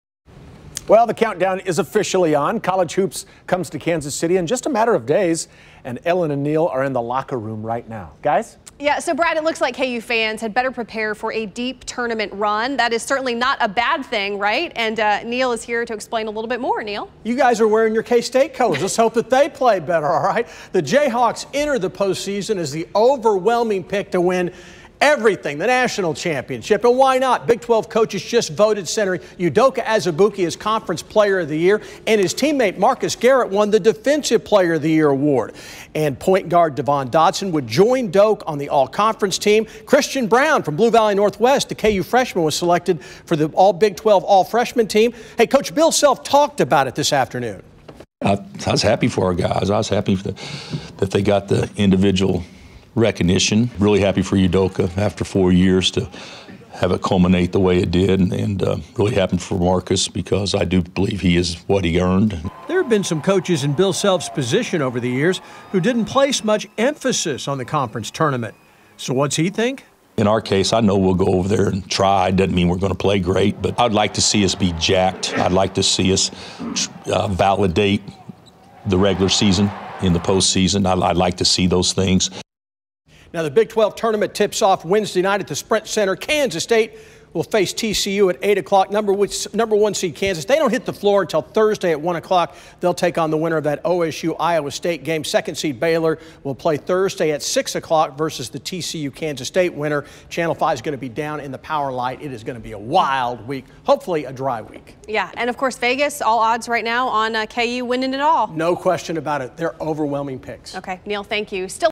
Bill Self talks Big 12 Awards and Tournament
The countdown is on! College hoops comes to town in a matter of days!